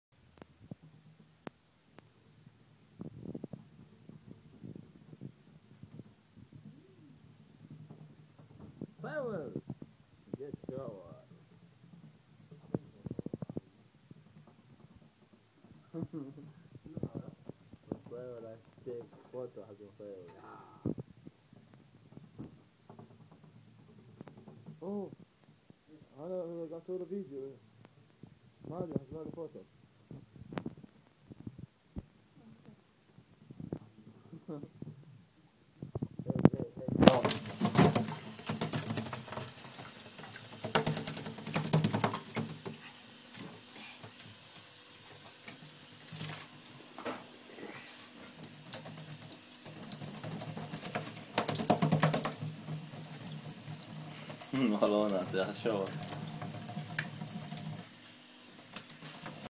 Lorikeet Dancing While Having a Shower
My Rainbow Lorikeet, Msewl (pronounced [em-se-will]) Having a shower in the sink. looks like he is enjoying it :D.